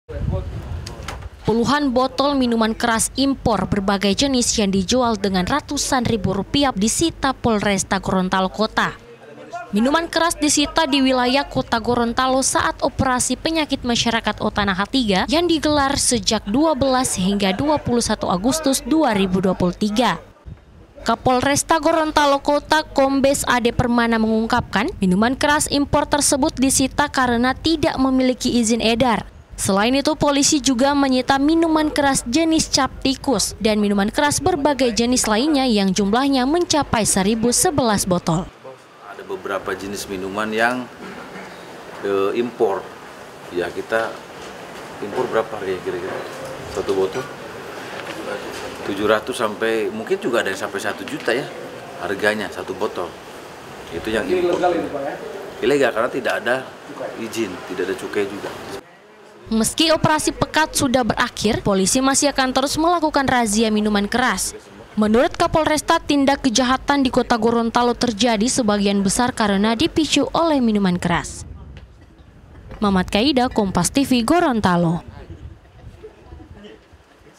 Tak Berizin, Miras Impor Berbagai Jenis Disita Polisi
KOTA GORONTALO, KOMPAS.TV - Puluhan botol minuman keras impor berbagai jenis yang dijual dengan ratusan ribu rupiah disita Polresta Gorontalo Kota.

Minuman keras disita di Wilayah Kota Gorontalo saat operasi penyakit masyarakat Otanaha 3 yang digelar sejak 12 hingga 21 Agustus 2023.

Kapolresta Gorontalo Kota Kombes Ade Permana mengungkapkan, minuman keras impor tersebut disita karena tidak memiliki izin edar.

Selain itu, polisi juga menyita minuman keras jenis cap tikus dan minuman keras berbagai jenis lainnya yang jumlahnya mencapai 1011 botol.

Baca Juga 8 Finalis Miss Universe Indonesia Minta Perlindungan LPSK, Kuasa Hukum: Mereka Dapat Intimidasi di

Meski operasi pekat sudah berakhir, polisi masih akan terus melakukan razia minuman keras.

Menurut Kapolresta tindak kejahatan di Kota Gorontalo terjadi sebagian besar karena dipicu oleh minuman keras.

miras impor

polresta gorontalo kota

operasi pekat

kota gorontalo